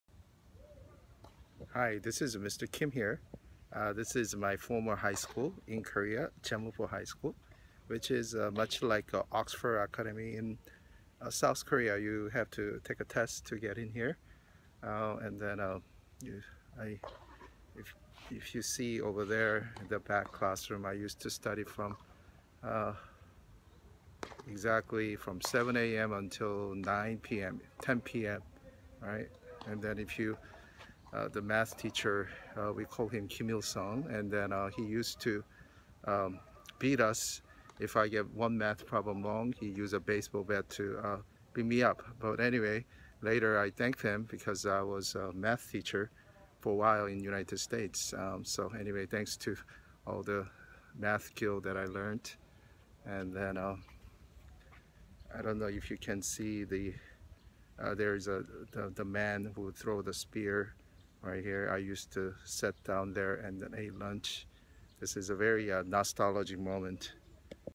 South Korea High School : 인천 제물포 고등학교. my former school Incheon South Korea
제물포고교, The school that I attended, one of the top-ranking high schools in South Korea. Many Korean celebrities and politicians and businessmen from this school.